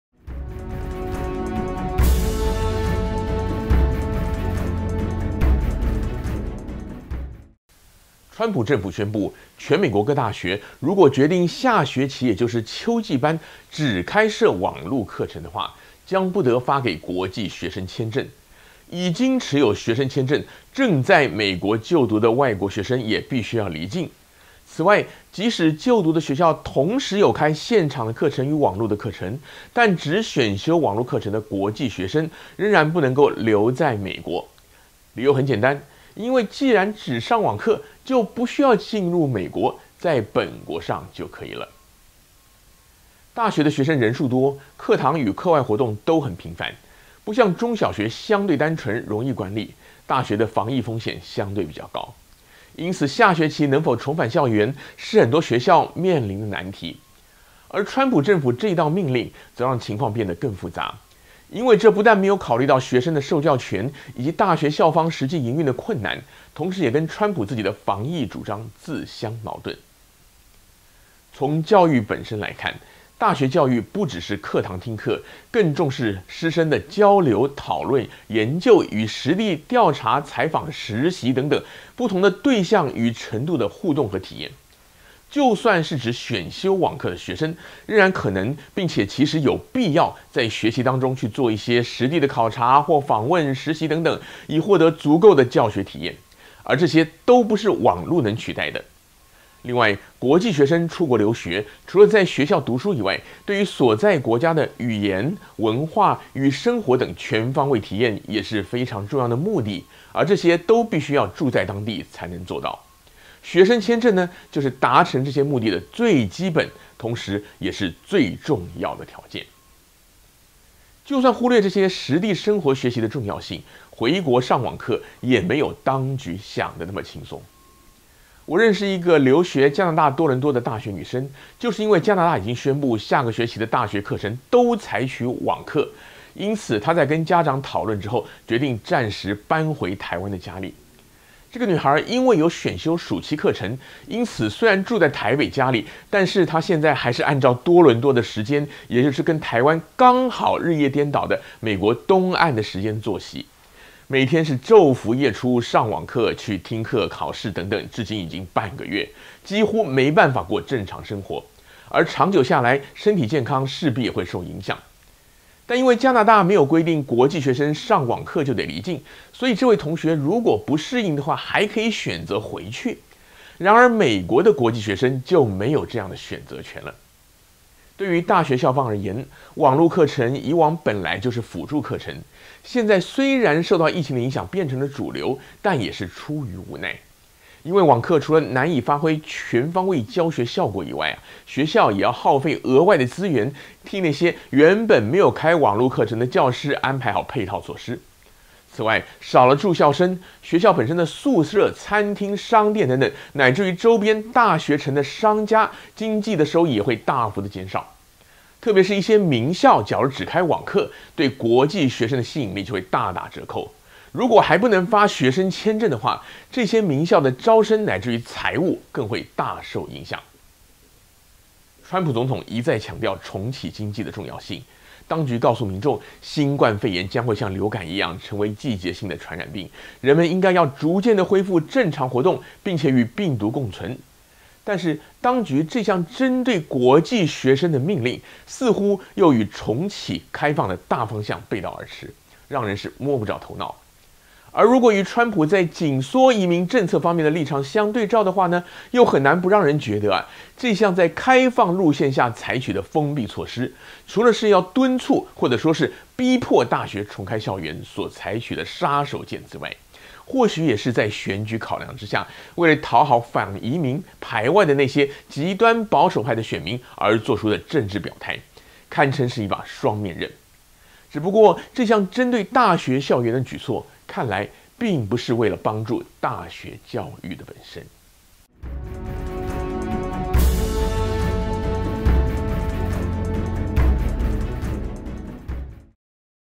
開放與封閉：大學抗疫的矛盾 焦點快評 20200707
川普政府宣布，全美國各大學，如果決定下學期也就是秋季班，只開設網路課程的話，將不得發給國際學生簽證，已經持有學生簽證，正在美國就讀的學生也必須離境。而這個命令不但沒有考慮學生的受教權，以及大學校方實際營運的困難，同時也跟川普自己的防疫主張自相矛盾...